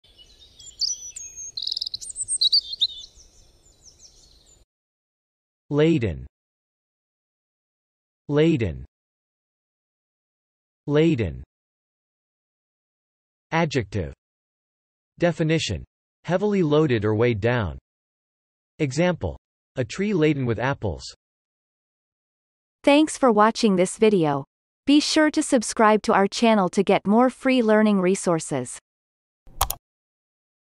How to pronounce laden - Vocab Today
The pronunciation of the word laden in Amercian accent is demonstrated in this video. If the word is from another language, such as brand name, it will be pronounced in its original language. Based on the word frequency, this is of 3 stars difficulty.

The word laden has the following meanings:
1. (adjective)
Definition : heavily loaded or weighed down.
Example : a tree laden with apples